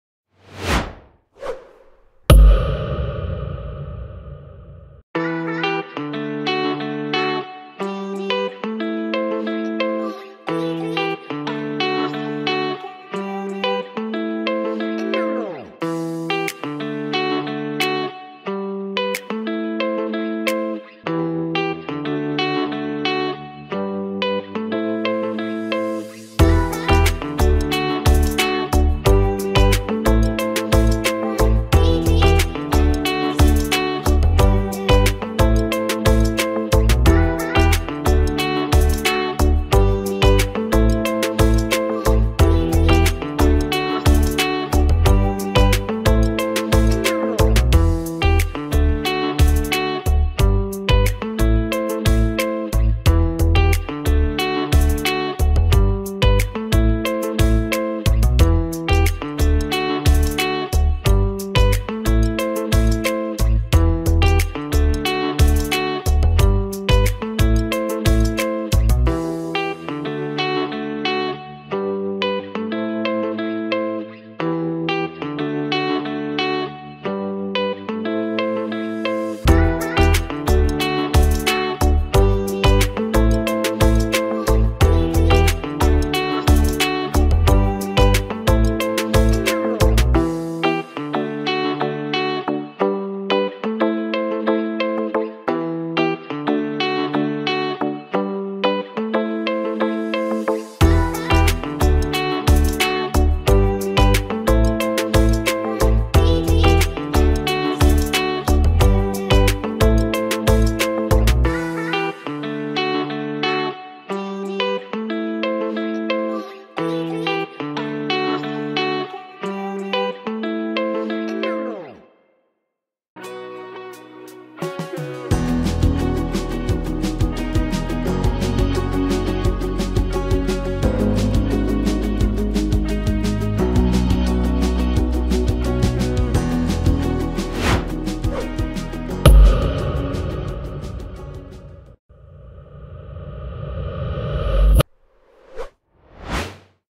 FES Trailer 2023
Unsere Schule aus Schülersicht - vielen Dank an Tim Hebisch aus der R8b!